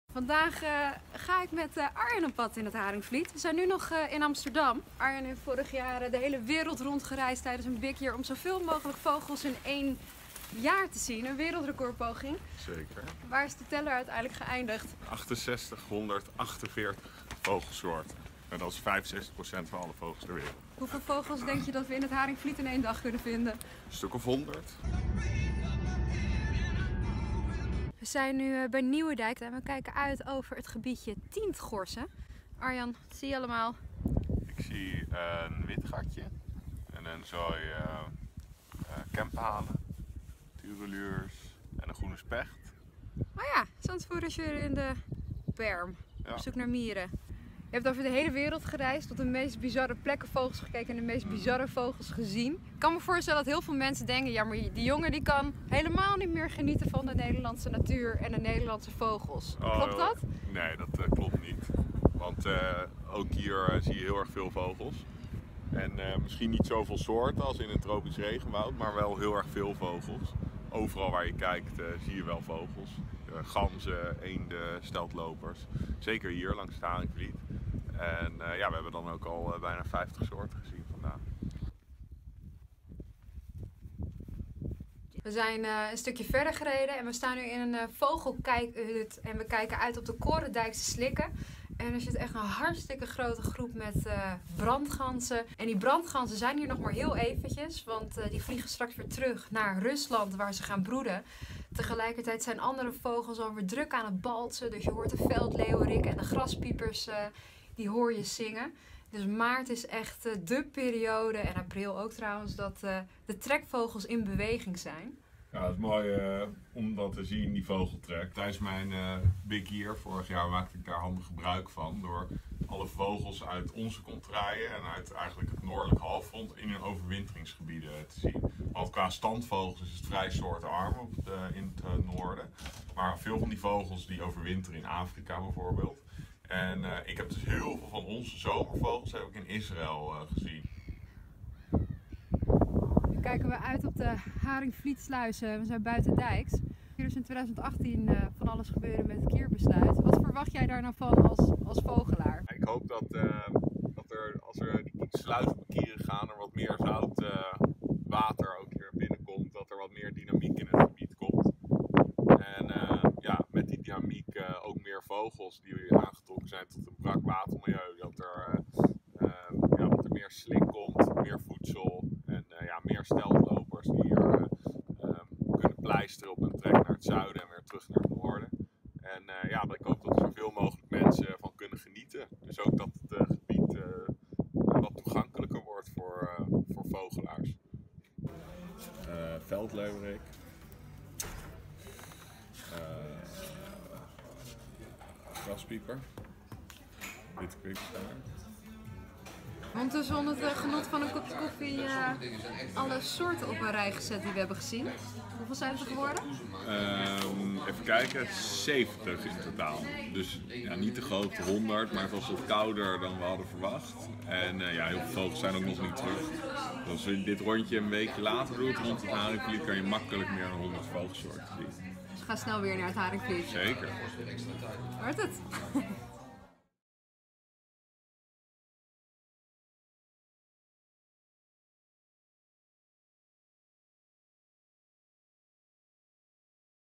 VLOG #16: Op stap met vogelaar Arjan Dwarshuis
Camilla is op stap met vogelaar Arjan Dwarshuis. In 2016 reisde Arjan de hele wereld over om zoveel mogelijk vogelsoorten in een jaar te zien. Tijdens dit zogenoemde "Biggest Year" zette hij het wereldrecord op 6841 vogelsoorten en  haalde geld op om bedreigde vogels wereldwijd te beschermen. Toch gaat hij nog graag vogels kijken in Nederland, zoals bij het Haringvliet waar de eerste trekvogels weer terugkomen uit het zuiden.